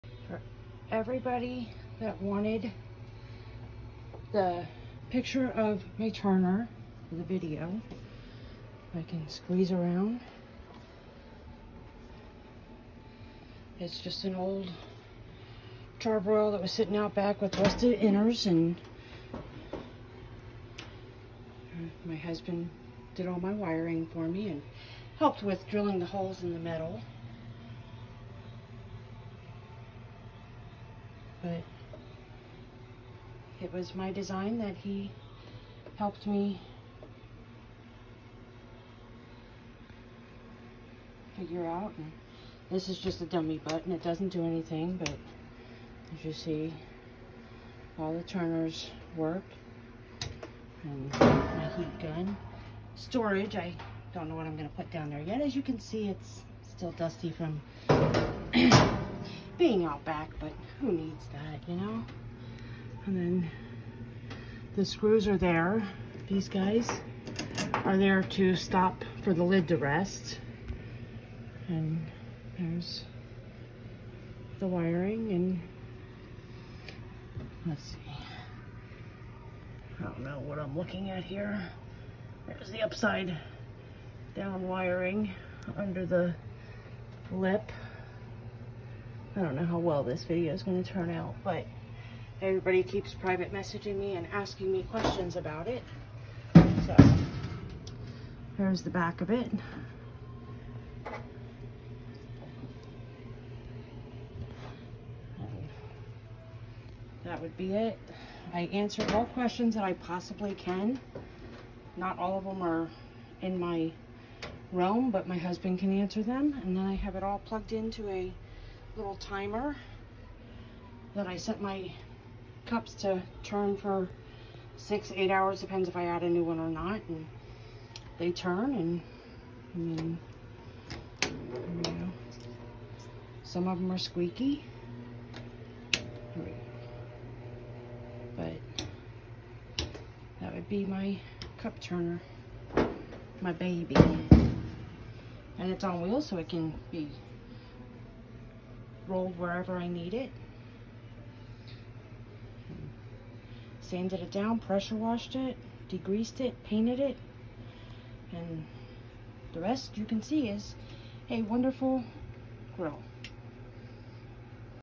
Upcycled My Old Grill to a DIY 5 Cup Tumbler Turner
Instead of adding to the landfill or shoving this beauty off to our local metal scrapper I had a vision.  My husband helped even though he had no clue what I was trying to do, he did as I asked and my baby was born.  There is nothing special on it.  The Grill was from Boscovs many years ago.  The PVC was stuff we had in the garage (being the owners of a 1951 house and all) the turner motors, light strips and switches were bought on Amazon.  The paint was Spray paints I have here for my tumblers.  Please don't hesitate to ask any questions.